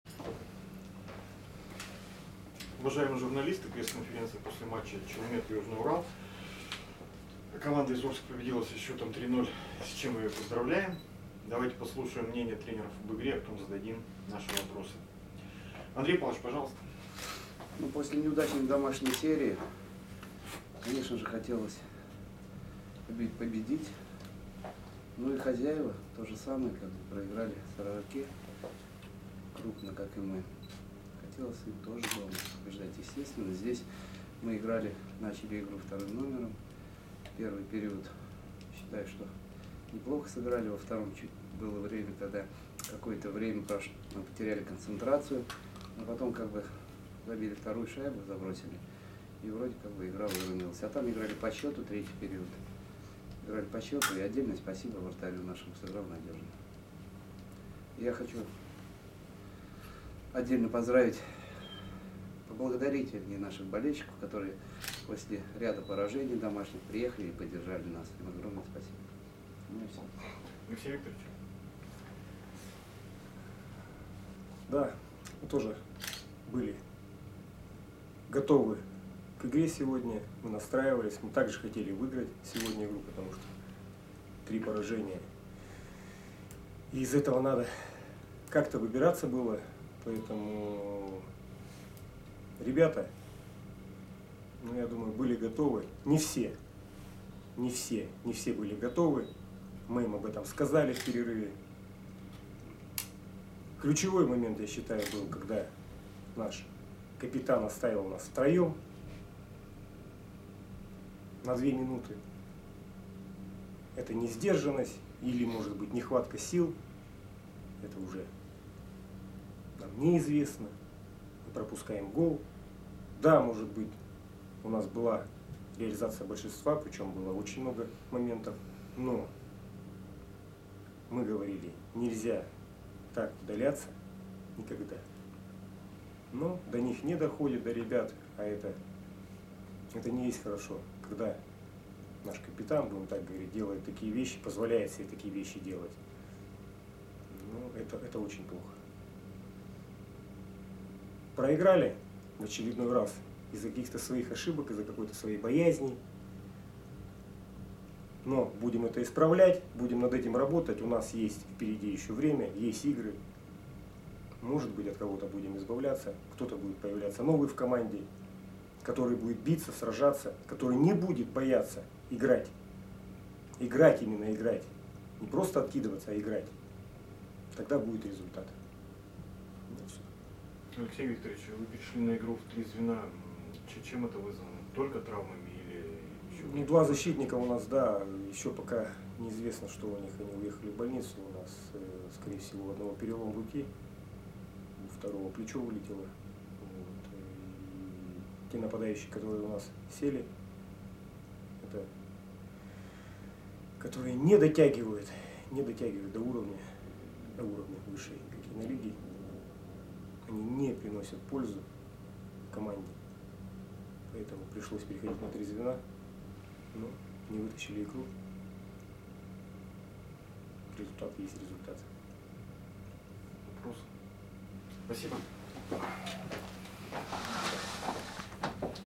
Андрей Кирдяшов и Алексей Тертышный
Пресс-конференция после матча Челмет (Челябинск) - Южный Урал (Орск) 0:3. 9 октября 2014